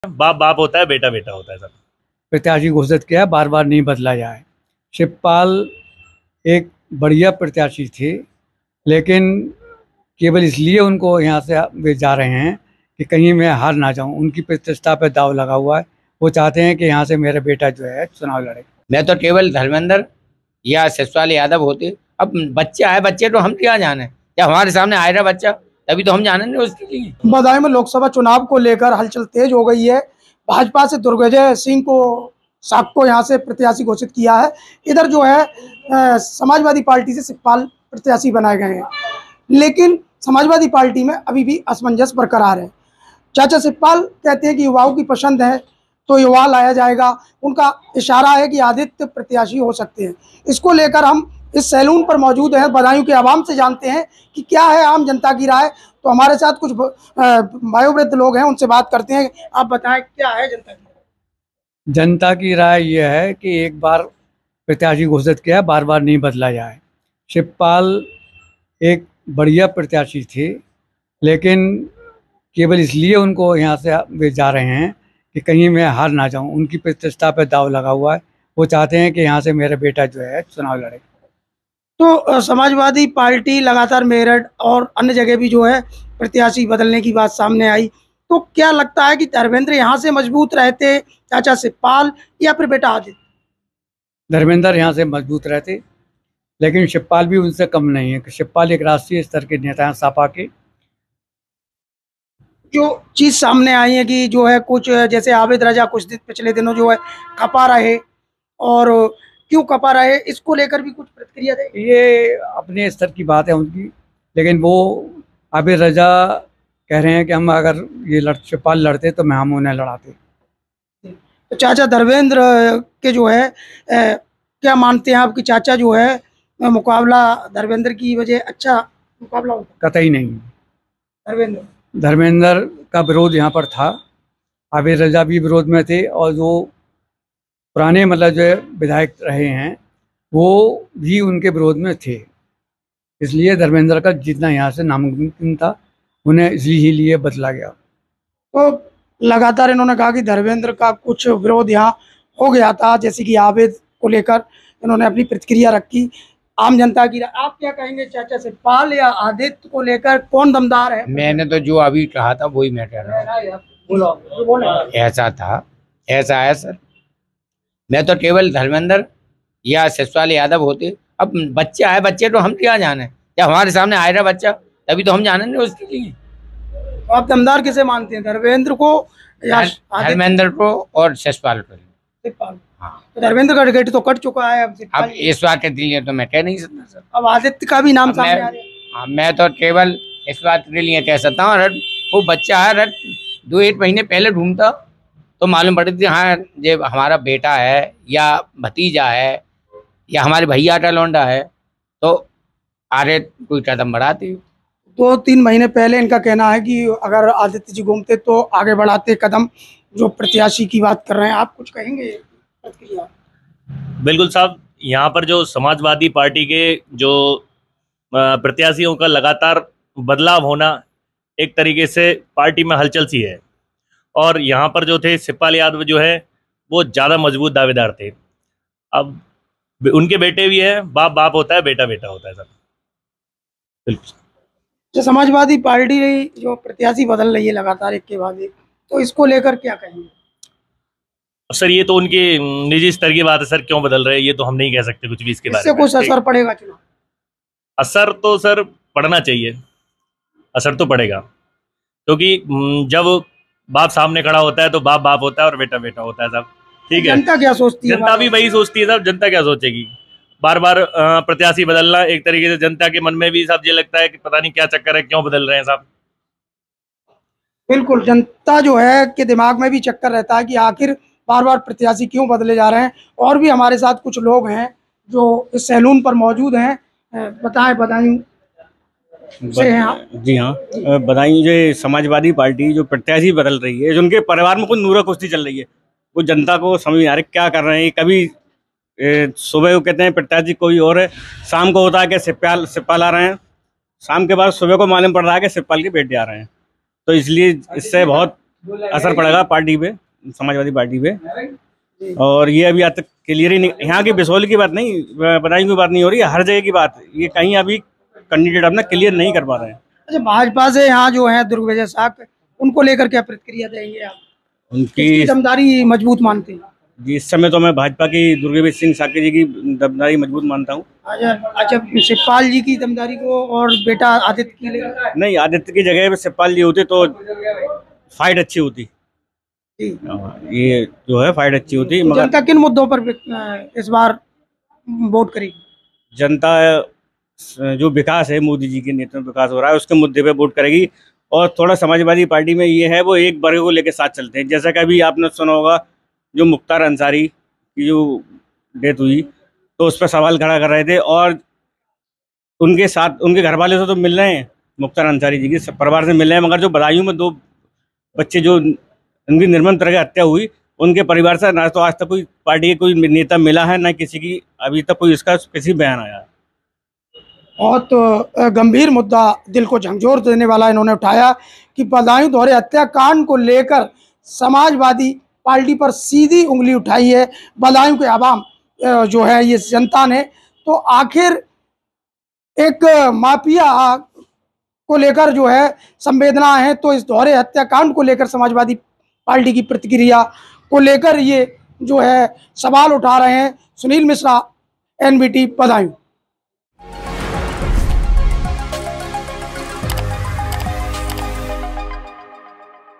Badaun Lok Sabha Constituency: Shivpal Yadav या बेटा Aditya Yadav, किसको चाहती है बदायूं की जनता ?
बदायूं के सैलून से जनता की राय समाजवादी पार्टी में प्रत्याशियों के बदलने के दौर जारी पर और शिवपाल,धर्मेंद्र ,आदित्य और भाजपा के दुर्विजय शाक्य में से कौन दावेदार मजबूत...मुख्तार अंसारी और अतीक पर समाजवादी पार्टी की संवेदना लेकिन बदायूं दोहरे हत्याकांड पर जनता ने खड़े किए सवाल जानिए क्या कहता है बदायूं का आवाम ।